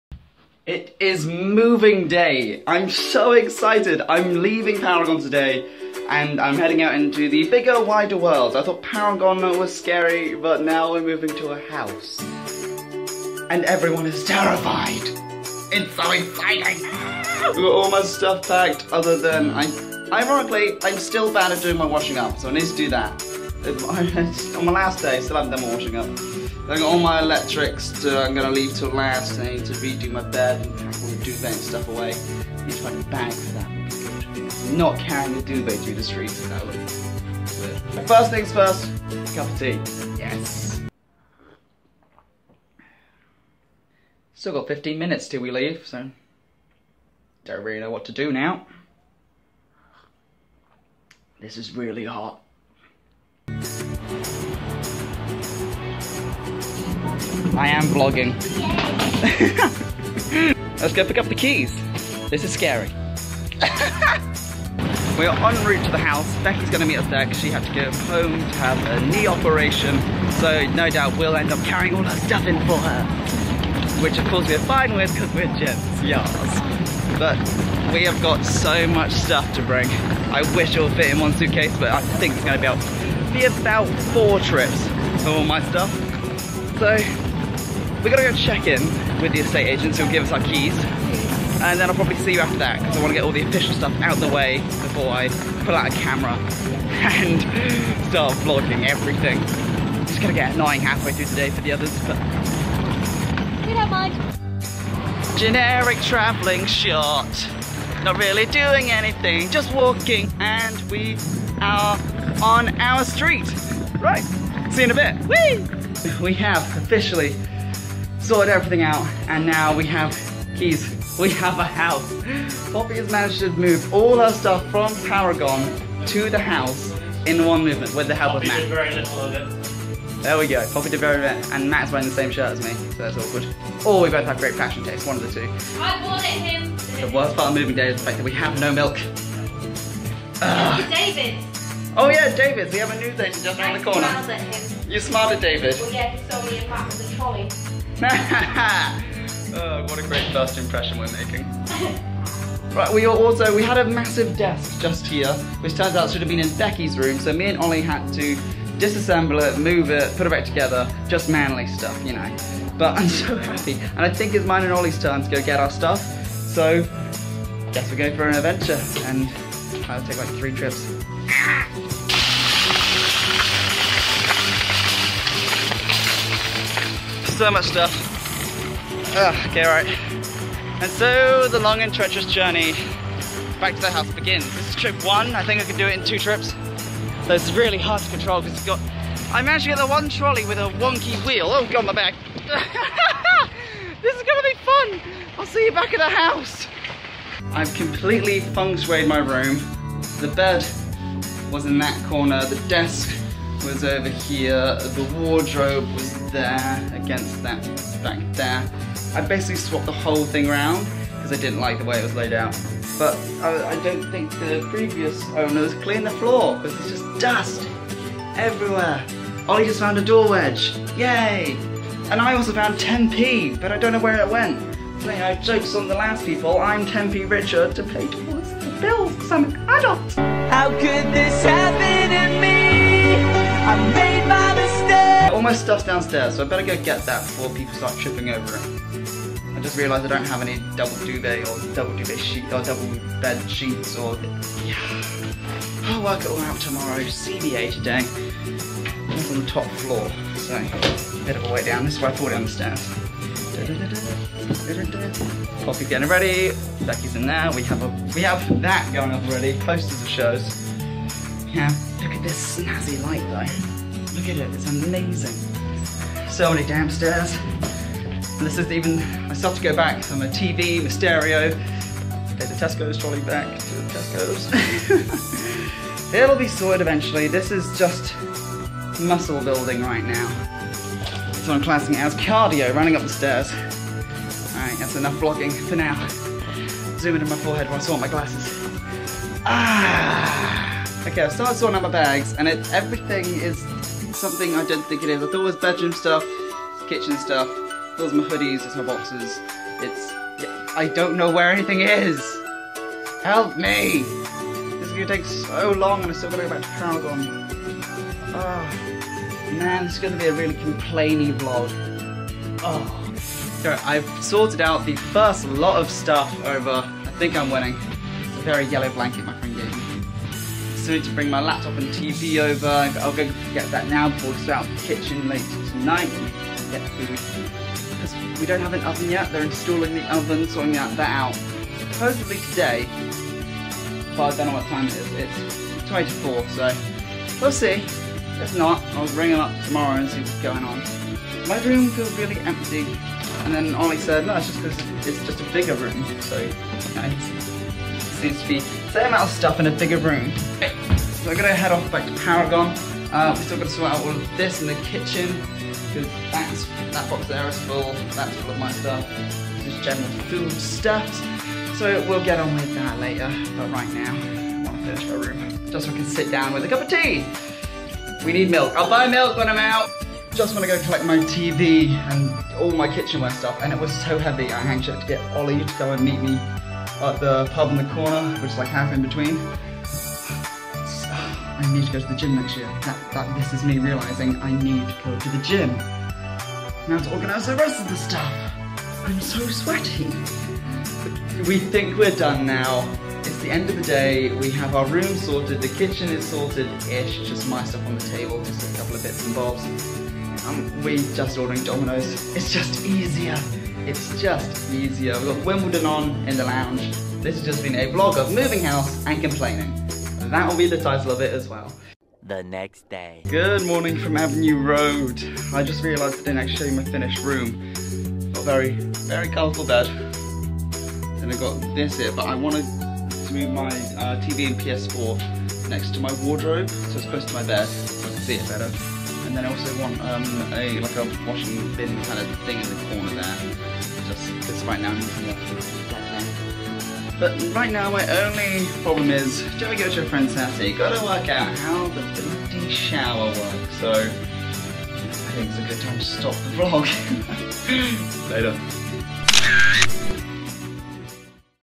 VLOG #9 || Moving Day!
Another step into being an adult male was taken today! Today, I moved out of my student accommodation and into a house with my friends ready for the second year of university!

And what is a better accompaniment so moving than complaining about moving? Enjoy!!!